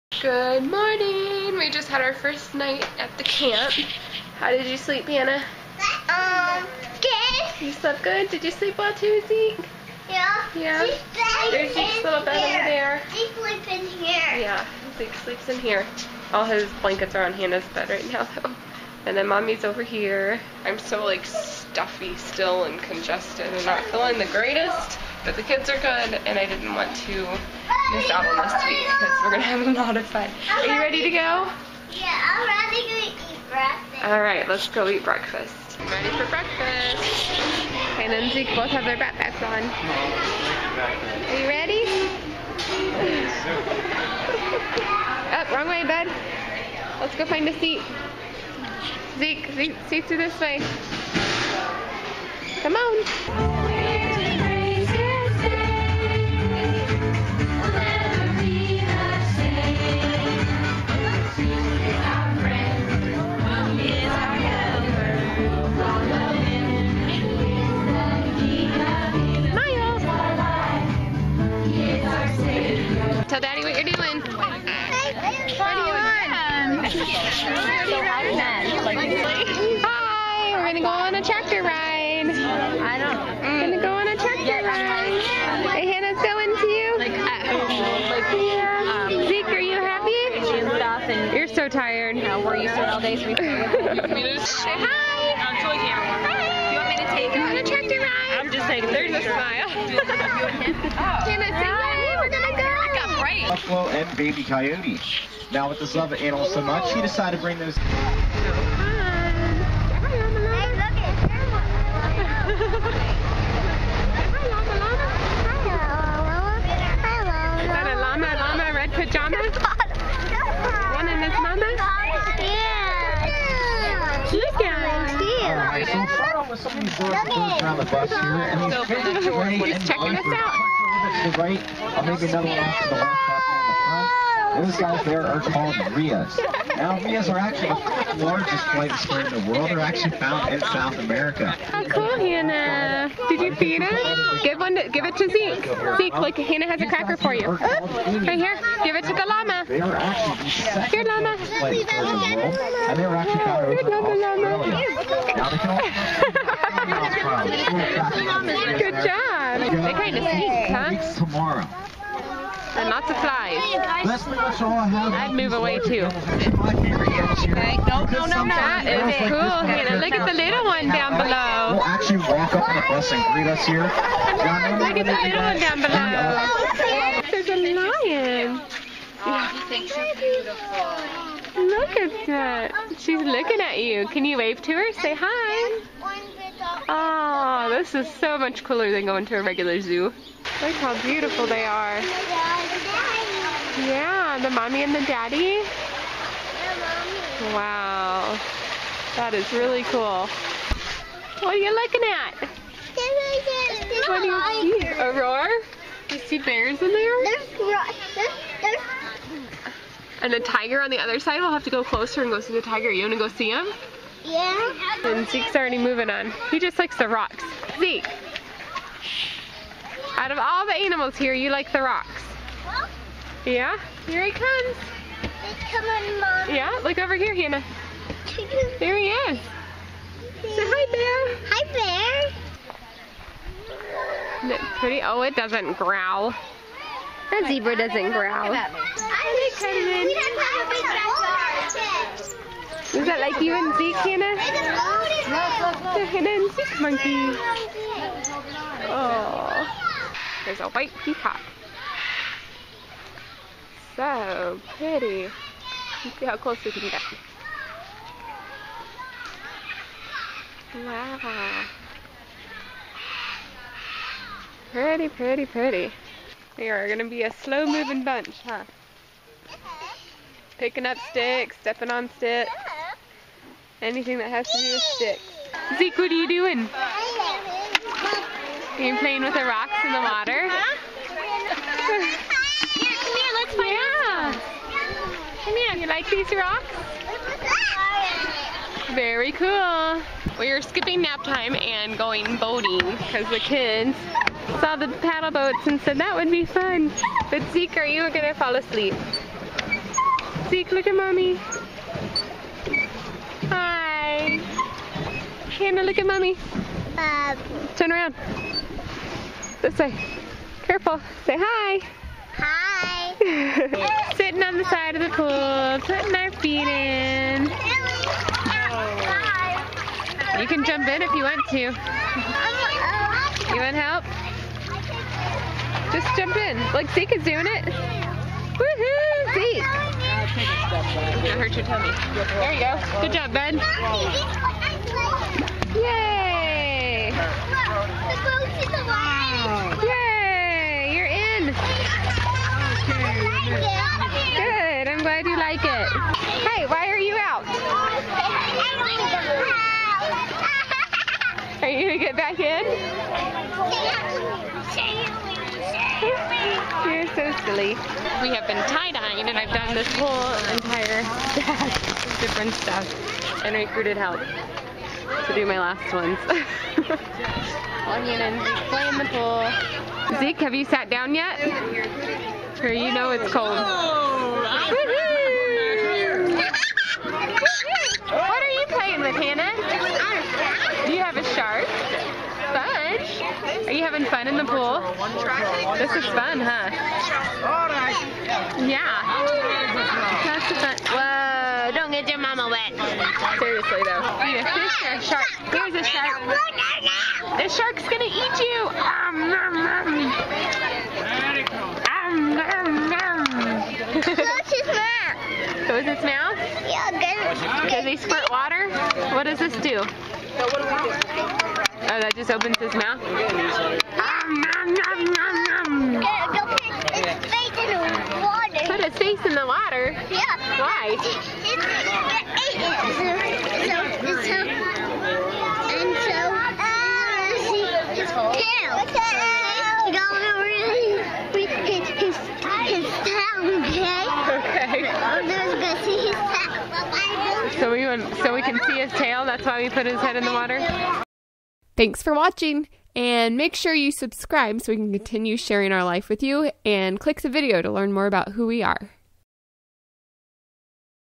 Wild Moms and Totts at camp!!
While I was at VidCon 2012, Dana took the kids to a Moms and Totts Camp. I missed them a lot, but they certainly had a lot of fun riding busses, looking at animals at a zoo, swimming, singing songs, paddle boats, creek stomping, and more! Definitely a great first camp experience for the kids.